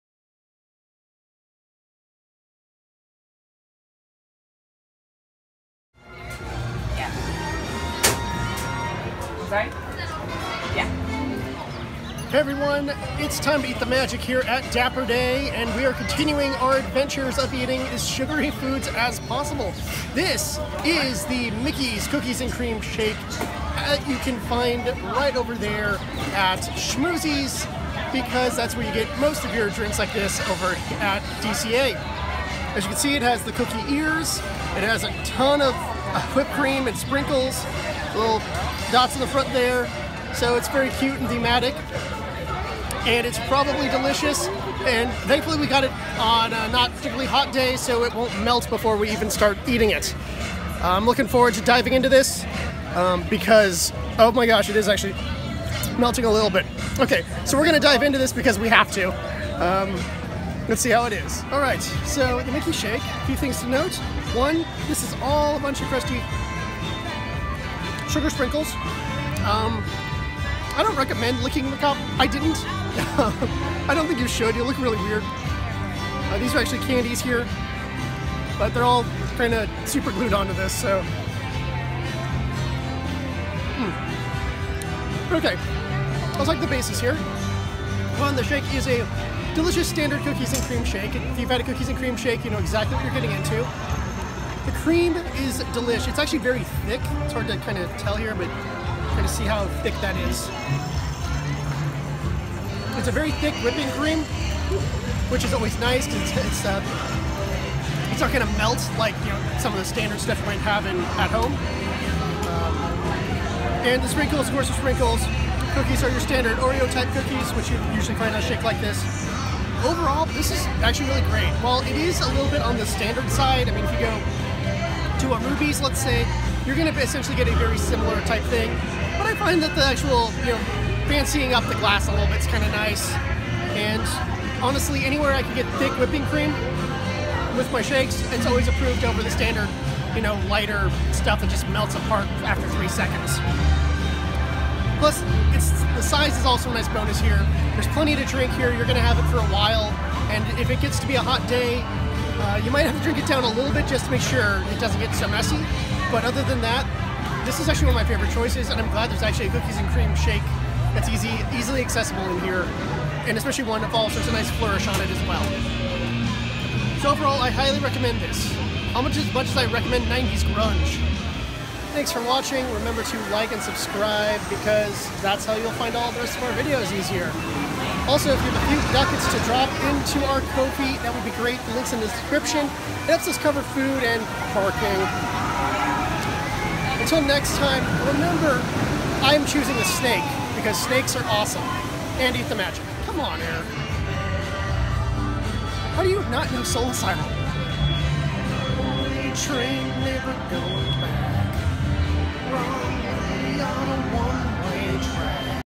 Eat the Magic 44 - Mickey’s Cookies ‘n Cream Milkshake
Get Your Ears On Challenge! We're trying all the Get Your Ears On items in 2019 to celebrate Mickey Mouse's 90th birthday. Looks instagramable, but tastes even better! Trying out the Mickey's Cookies 'n Cream Milkshake from Schmoozies!